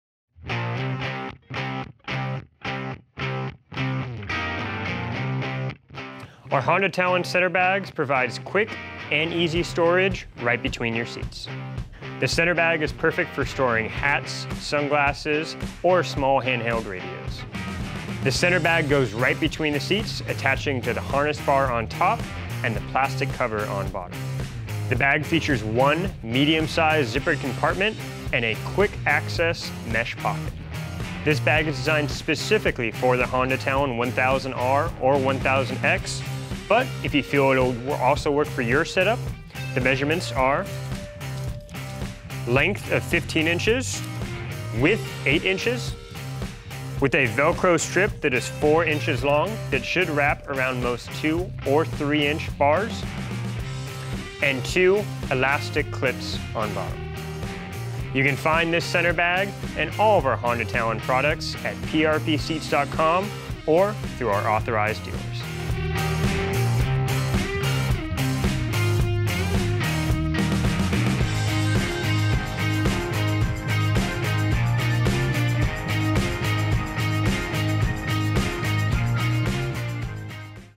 PRP Center Bag for Honda Talon
PRP’s Center Bag for Honda Talon is designed to hang between the seats, and give you easy to access storage within arms reach. Its perfect for holding extra water bottles, hats, sunglasses and much more. Includes an external mesh pouch for quick access.

Fits the Honda Talon 1000X and 1000R. Secured to the vehicle with two loops on top that wrap around the harness bar, and two hooks on the bottom that attach to the access panel. Made from durable vinyl-coated nylon and marine-grade vinyl, with rubberized PRP zippers to reduce noise. Measures 15″ x 8″ x 2″. Sold Individually.